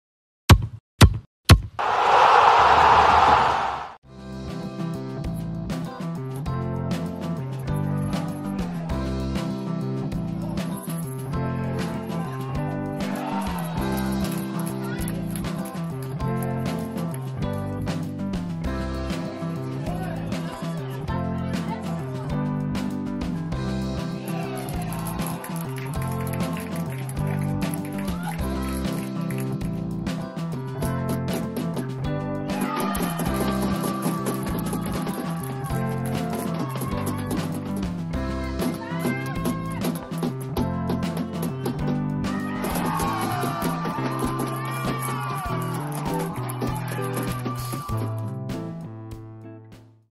TJ Dražice – TJ Blaník Strunkovice 2:2
Sestřih gólů z utkání 7. kola KP Dražice vs. Strunkovice.